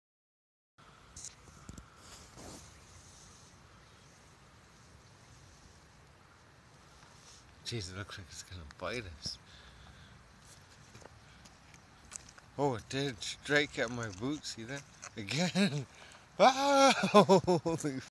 Snake attack in Gitwinksihlkw
Brown snake bit my boot